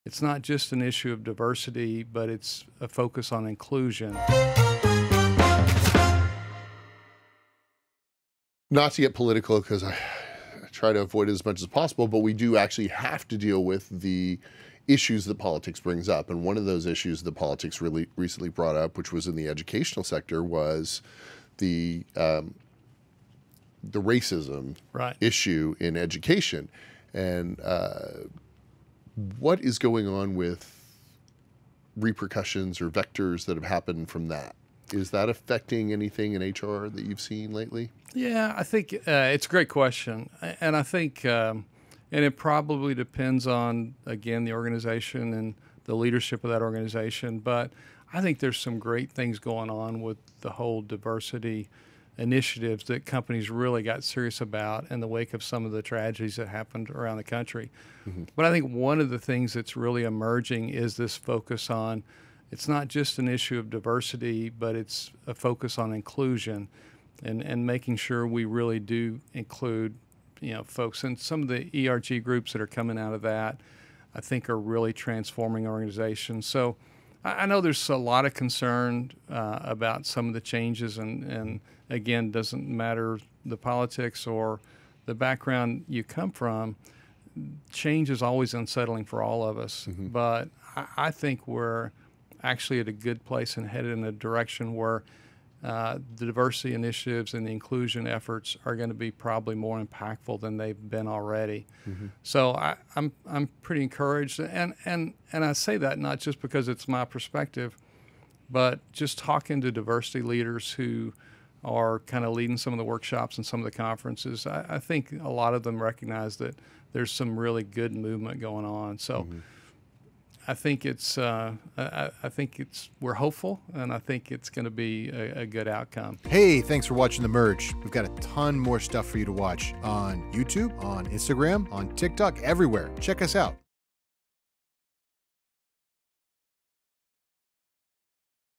HR's Response to Systemic Racism
Today's workforce is no longer just about diversity. It's about inclusivity. But how do we drive that change and successfully embrace everyone?

Always do your own due diligence before making investments.